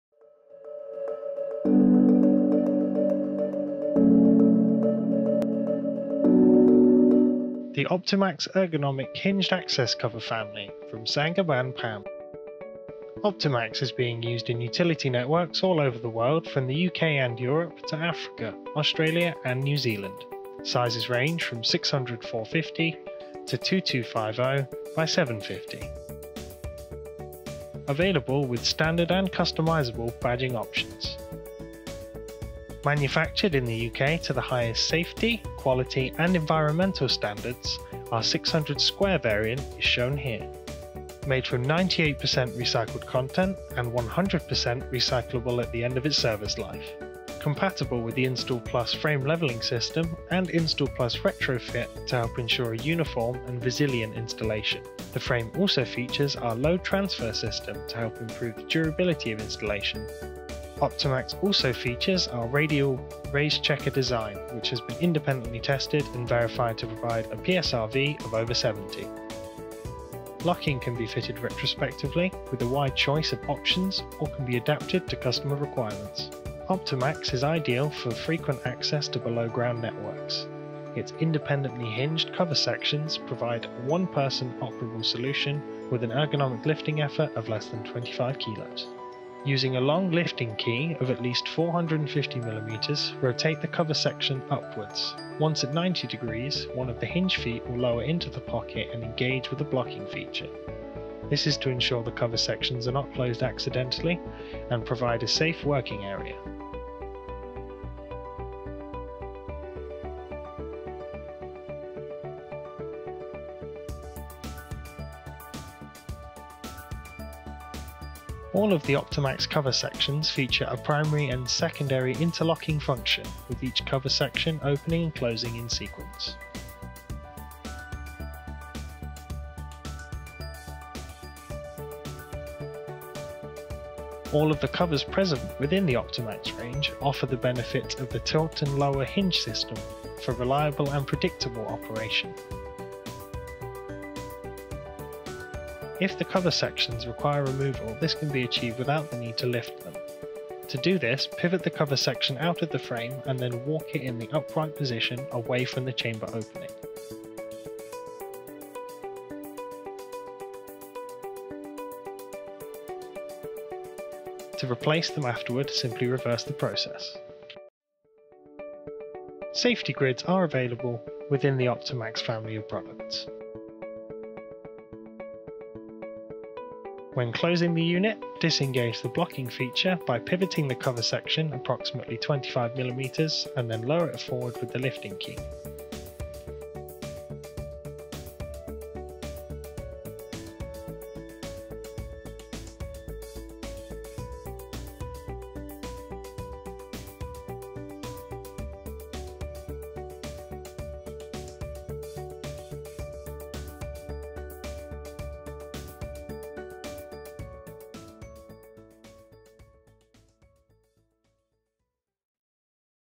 Opt-Emax Features & Benefits
Available in a wide range of clear openings and in 100mm or 150mm frame depths, Opt-Emax is the ergonomic solution for the frequent maintenance and inspection of below ground assets, and has become the product of choice for the Highway Communications, Water and Telecom sectors. Opt-Emax includes many features that combine ease of use, safety, security and durability giving clients, specifiers and operators a versatile solution for a wide spectrum of applications.